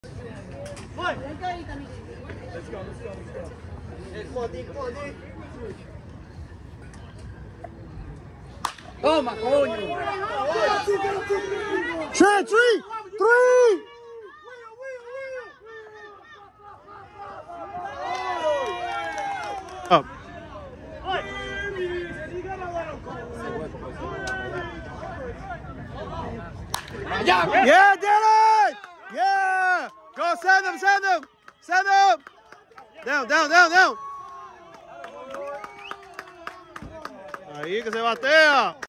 2022 16U PG NORTHEAST FALL WORLD SERIES
Dylan Espinal (NJ, Class of 2026) playing at PG 16U Northeast Fall World Series. 2 ABs, 2-2, 2B and RBI Single.